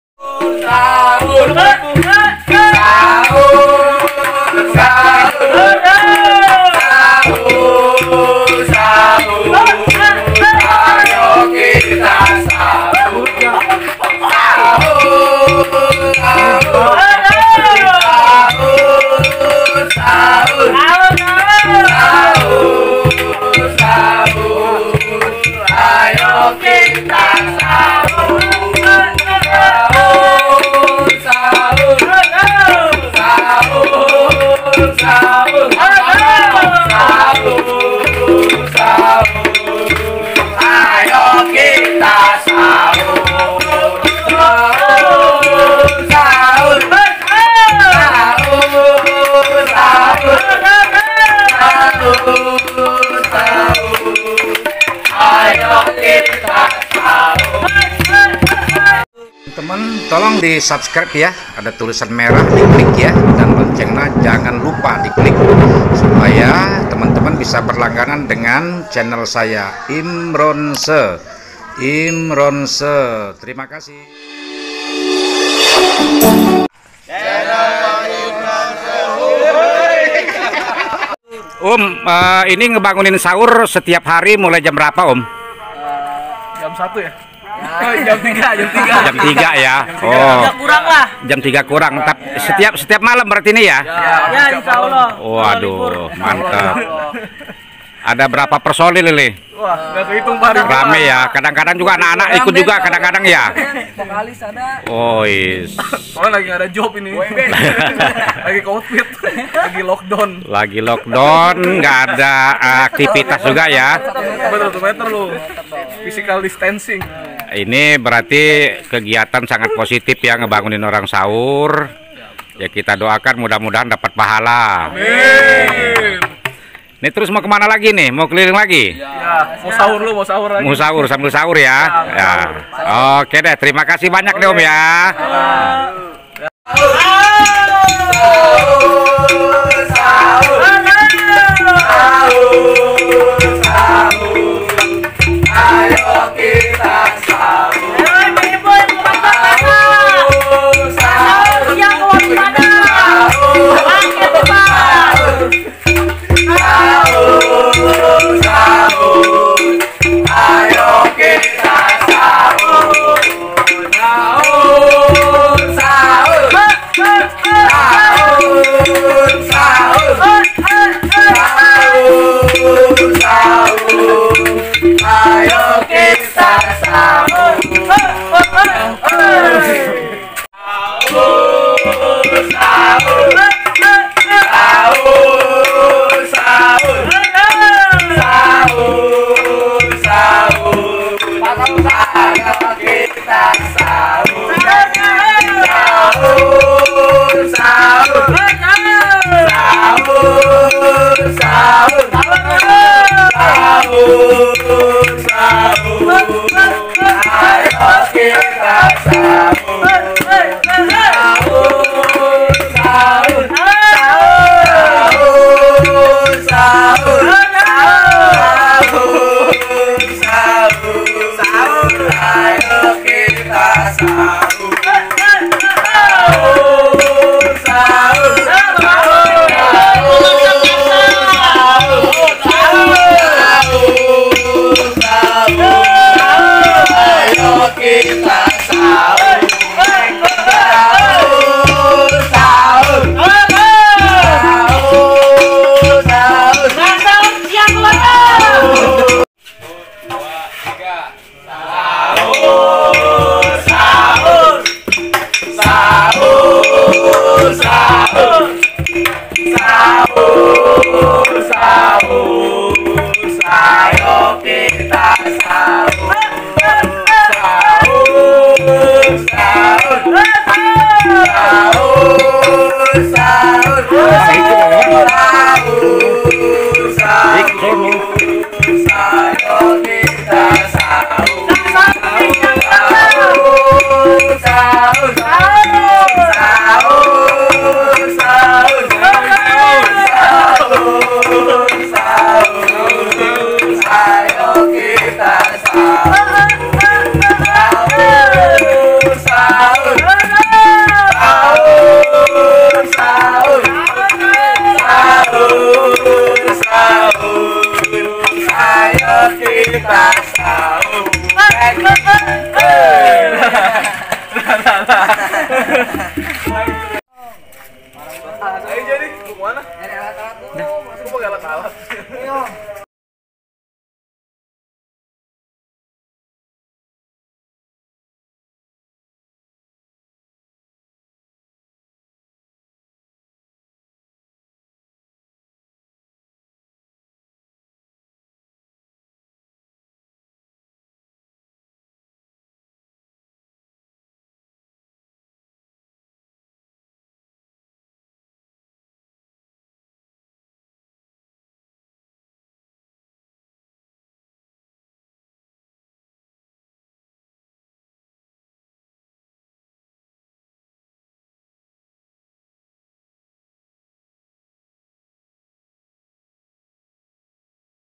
KREATIFITAS REMAJA MEMBANGUNKAN SAHUR - imron se
Kreatifitas Remaja membangunkan Sahur merupakan tradisi nenek moyang dahulu, tradisi yang sangat positif, tapi memang ini merupakan salah satu keunikan yang kita temukan dalam bulan Ramadhan. Apalagi dilakukan oleh anak Remaja, sangat langka dan sangat membantu warga.